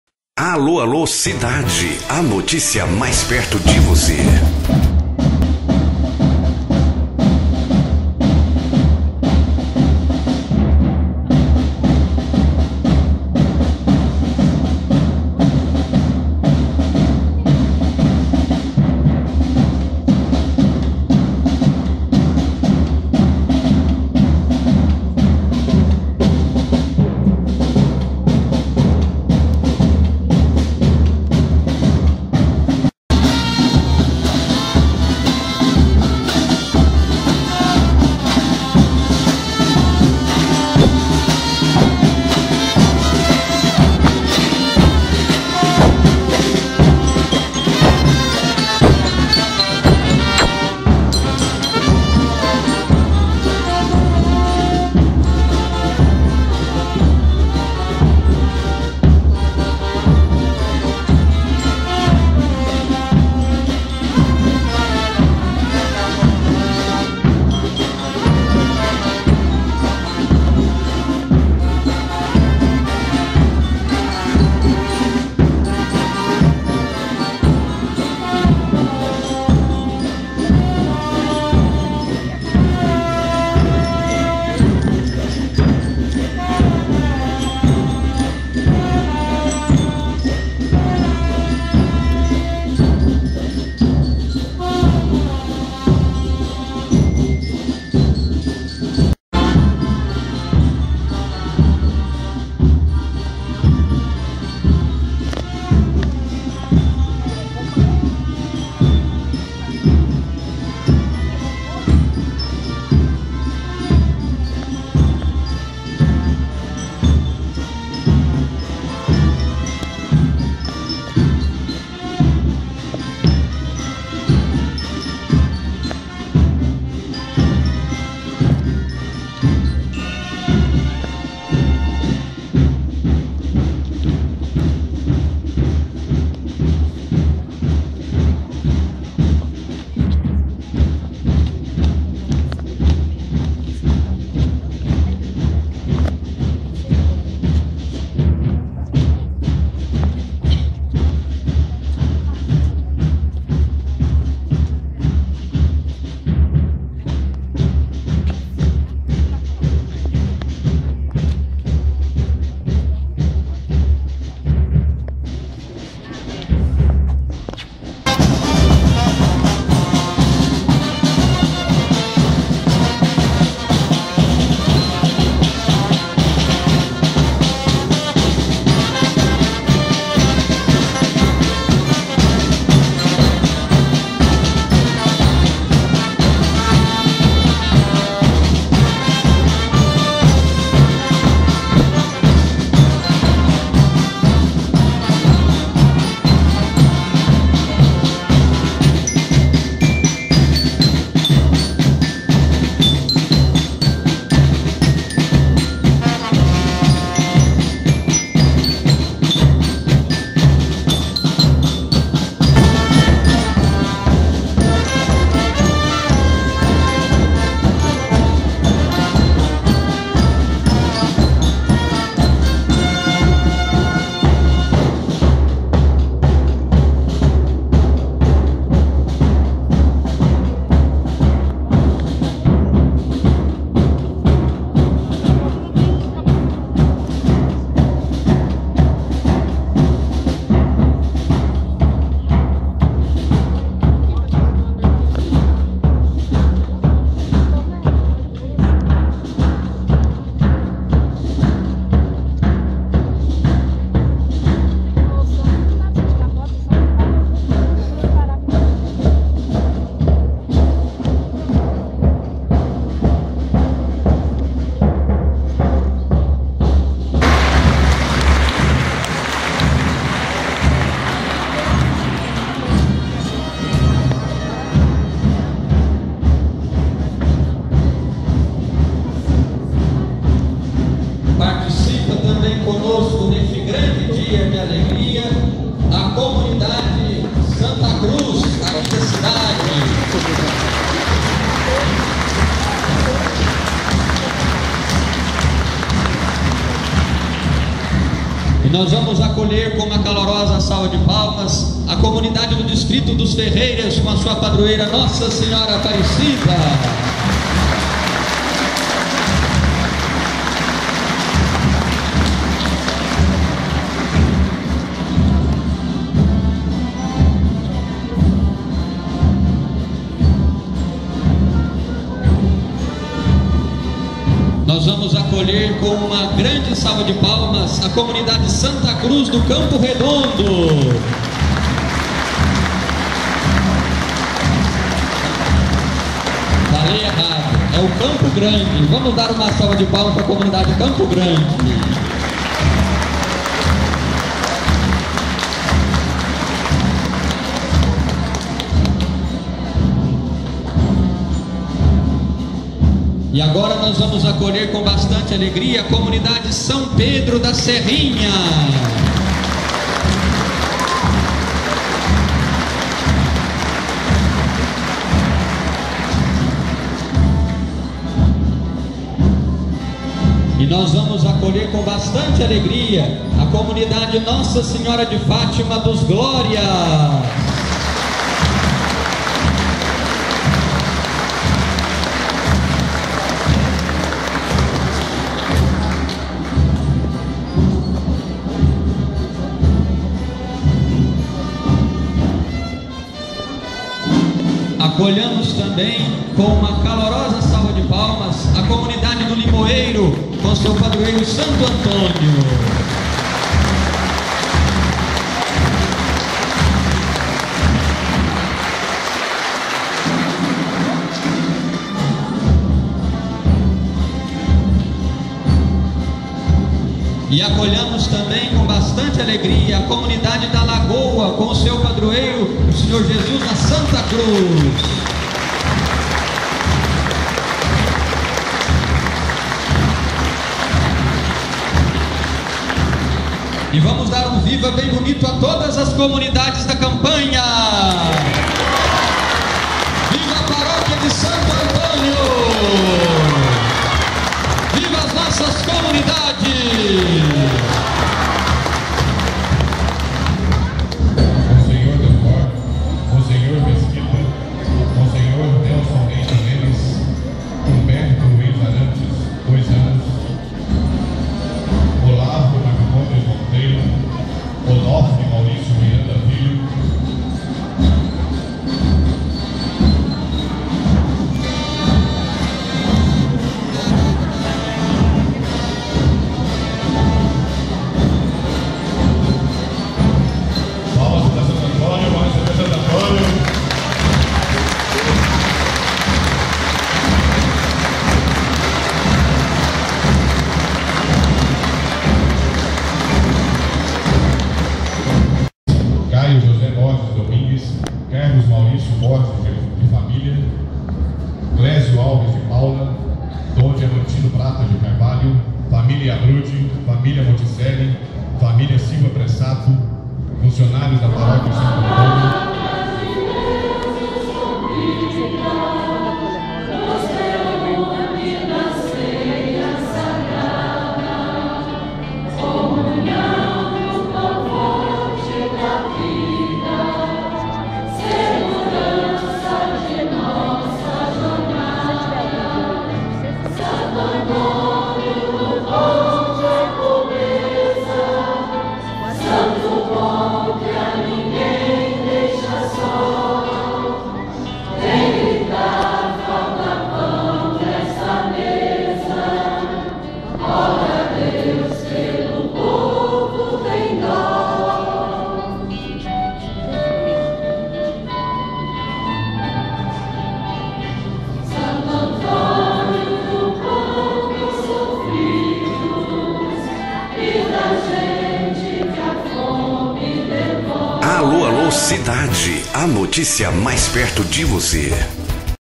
Procissão dia de Santo Antônio em Campanha, MG 13/06/2014
Procissão, teve a presença de várias comunidades da cidade. Muita fé.